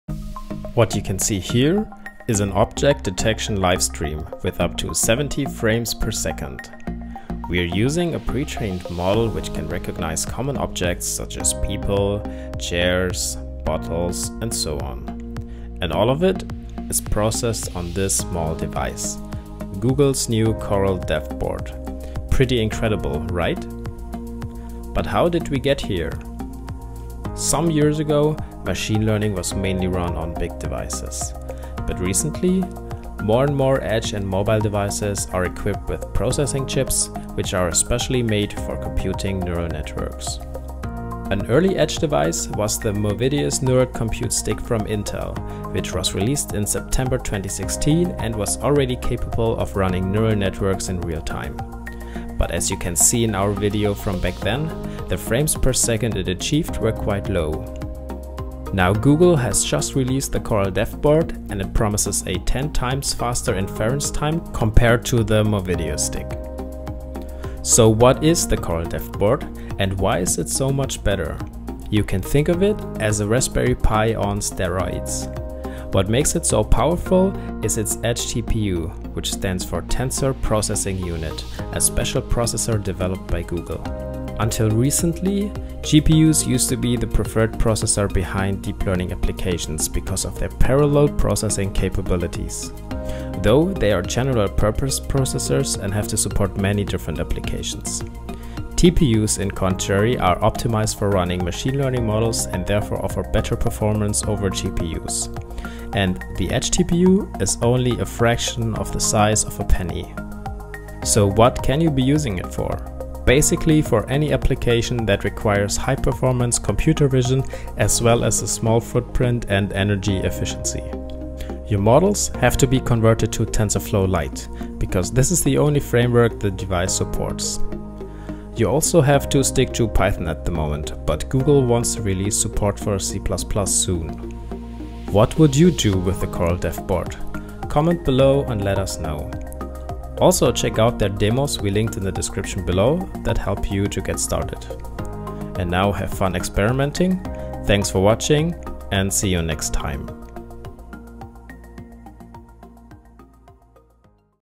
Google's Coral Dev Board with Edge TPU - realtime object detection at 70fps
Realtime object detection with Google's Coral Dev Board with Edge TPU at 70fps.

Author: Paul Strobel

UPDATE: C++ is now supported